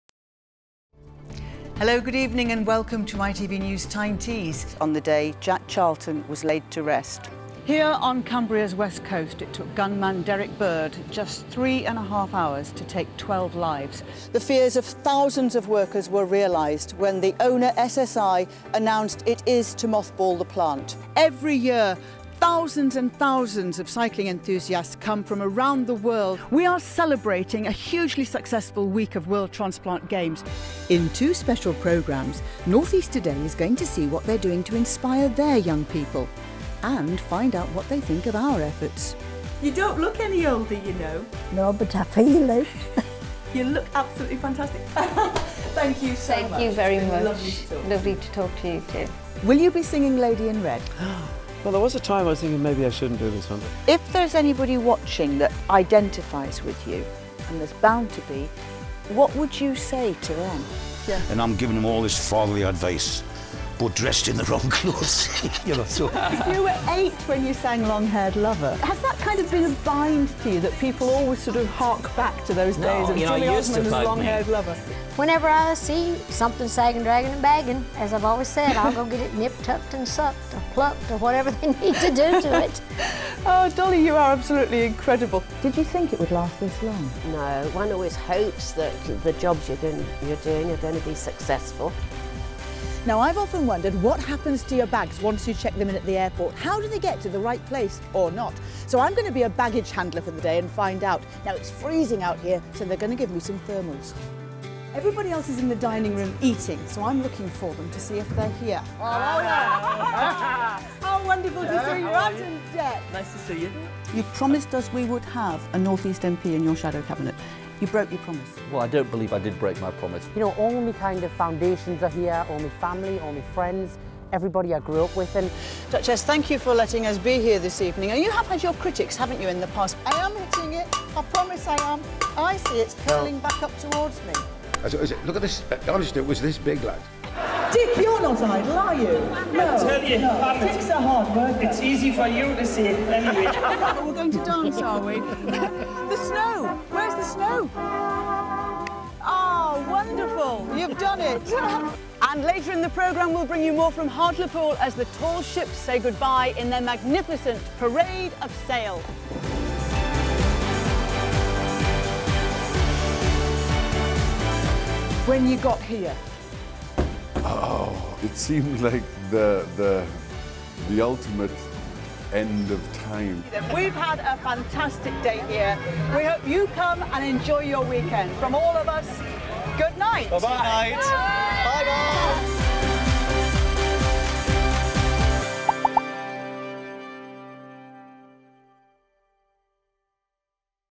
Pam Royle - Showreel
This is Pam Royle's showreel. Pam is a British television journalist and presenter.